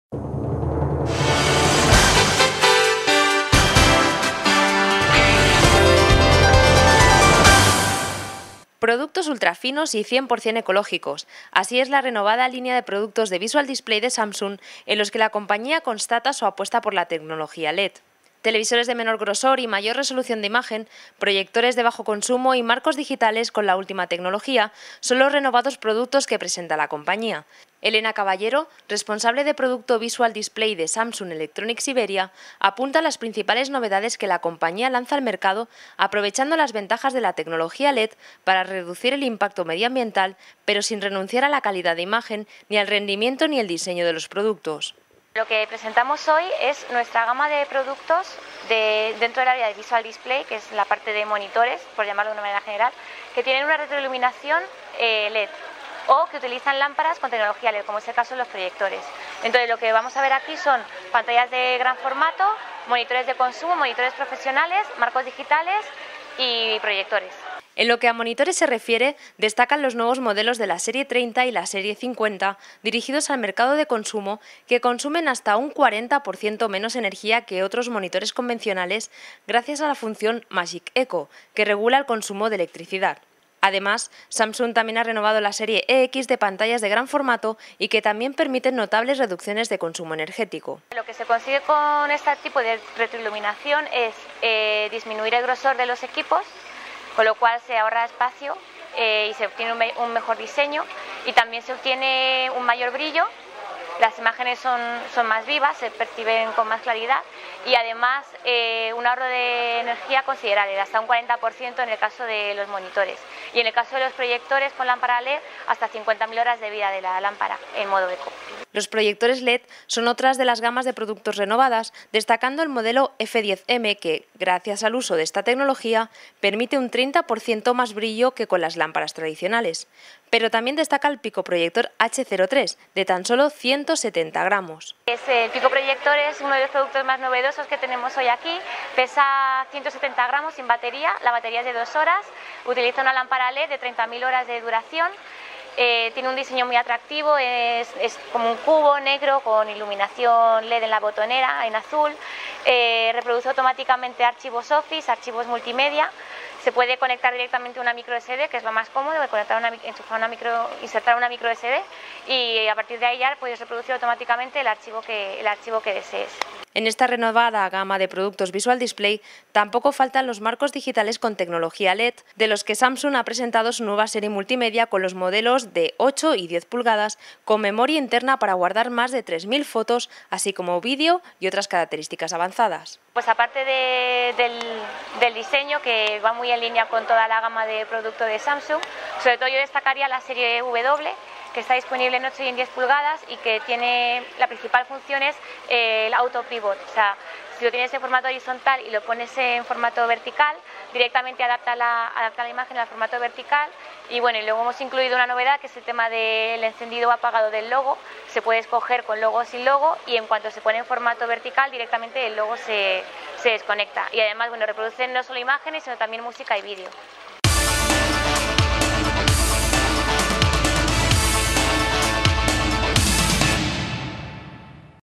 Samsung renueva su gama Visual Display
Samsung renueva su línea de productos Visual Display con tecnología LED. Elena Caballero, responsable de producto Visual Display en Samsung Electronics Iberia, muestra las ventajas que esta tecnología permite para sus renovadas pantallas, proyectores y marcos digitales.